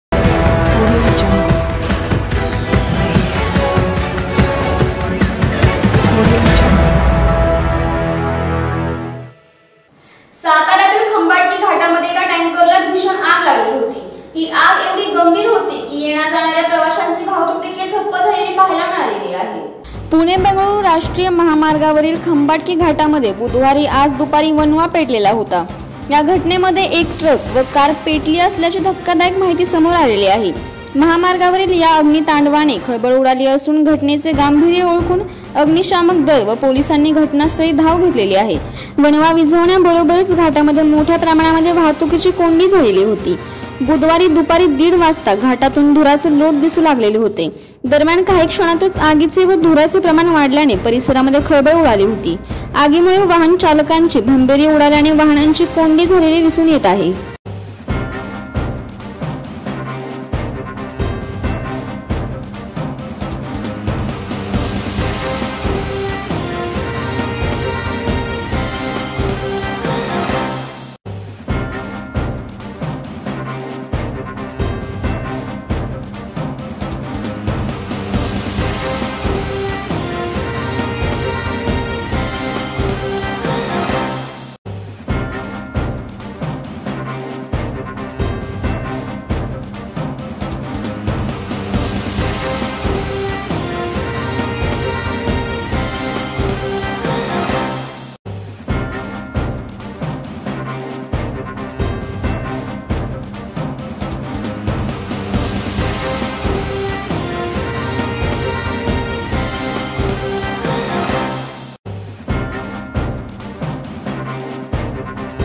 पुणे-बंगळूर राष्ट्रीय महामार्गावरील खंबाटकी घाटात वणवा - एक ट्रक व कार पेटली
बुधवारी दुपारी दीड वाजता घाटातून धुराचे लोट दिसू लागले. दरम्यान, काही क्षणातच आगीचे व धुराचे प्रमाण वाढल्याने परिसरात खळबळ उडाली. आगीमुळे वाहन चालकांची भंबेरी उडाल्याने वाहनांची कोंडी झाली आहे.